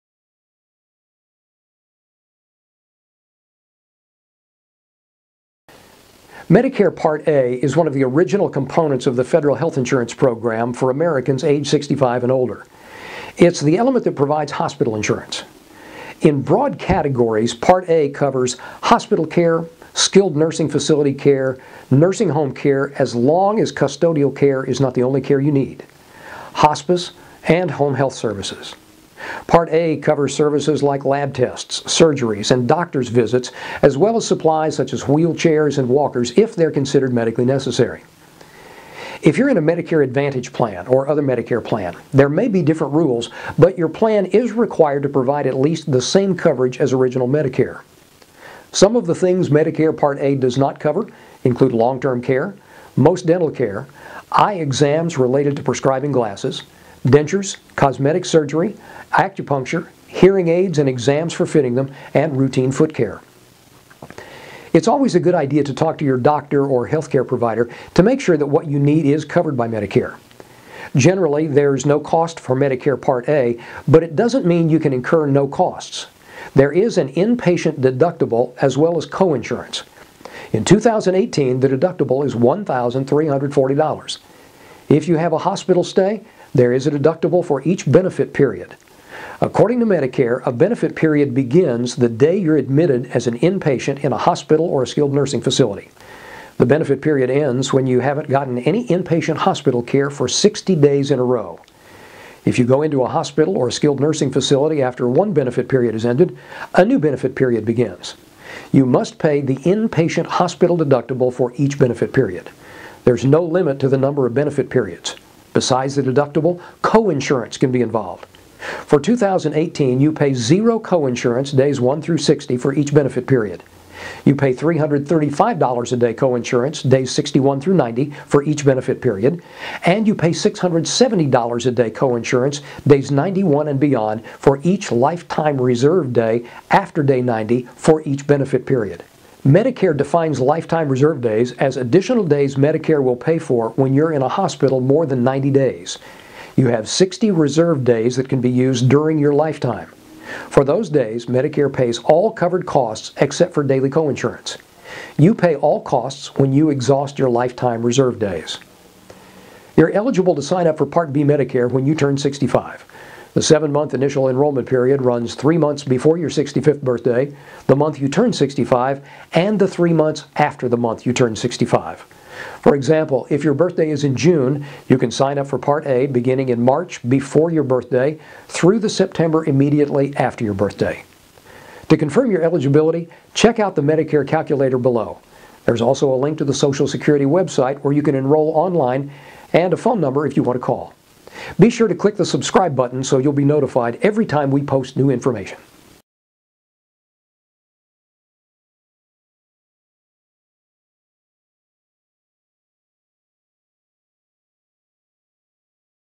Medicare Part A
Understanding Medicare Part A - Hospital Insurance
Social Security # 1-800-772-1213
Social Security website www. medicare.gov